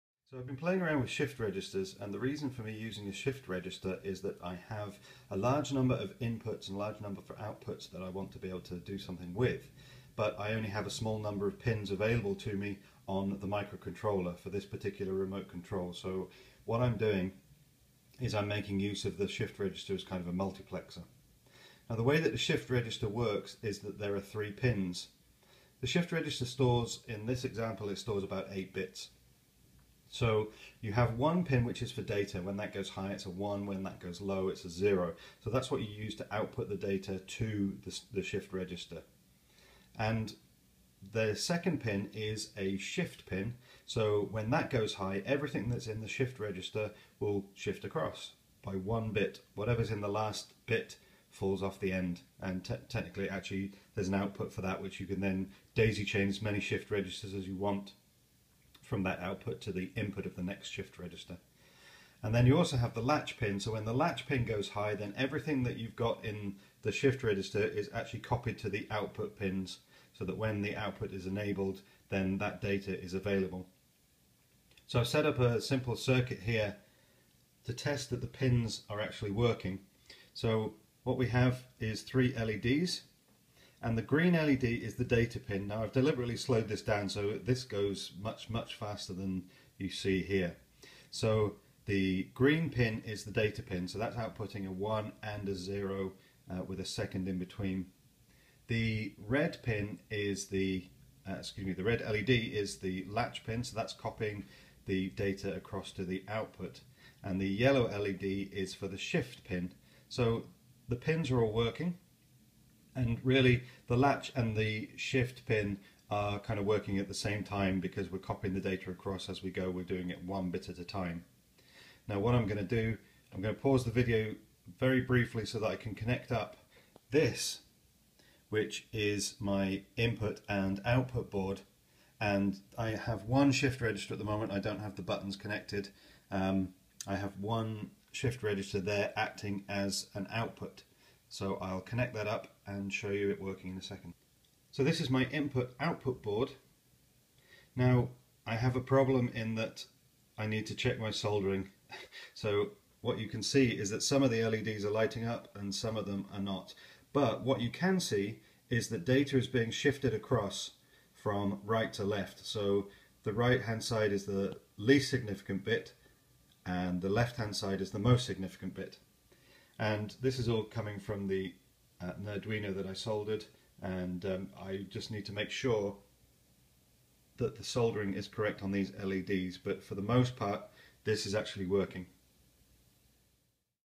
Roombaduino - part 5 - Soldered Shift Registers
Showing the soldered Nerduino "working" with the soldered IO board